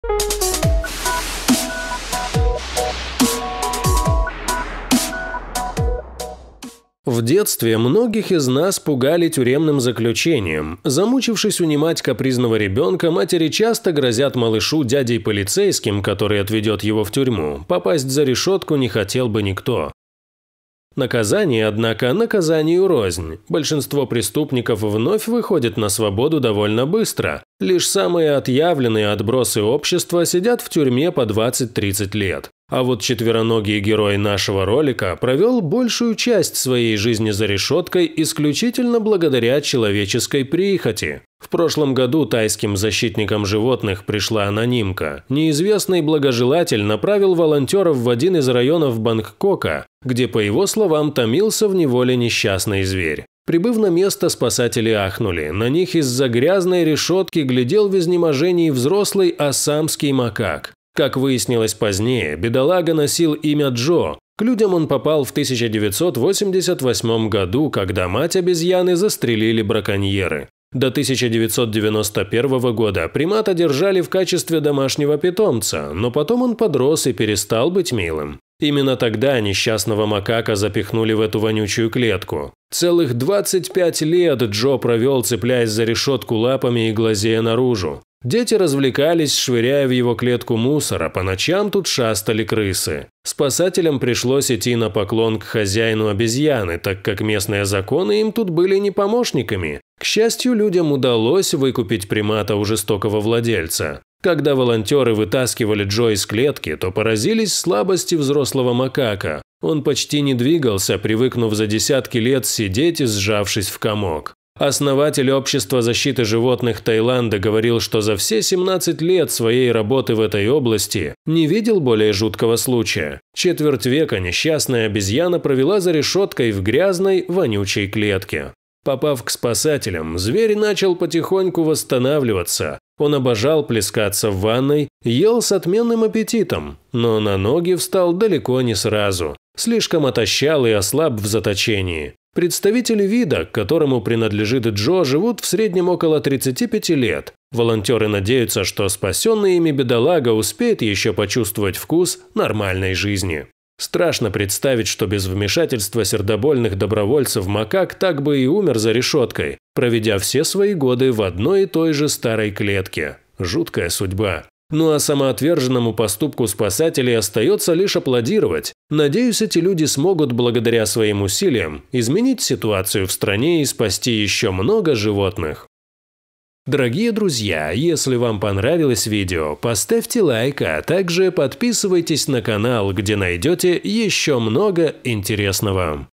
Прибыв на помощь к животному, волонтеры ужаснулись. Из вонючей дыры в стене к людям потянулась рука…
В детстве многих из нас пугали тюремным заключением. Замучившись унимать капризного ребенка, матери часто грозят малышу дядей полицейским, который отведет его в тюрьму. Попасть за решетку не хотел бы никто.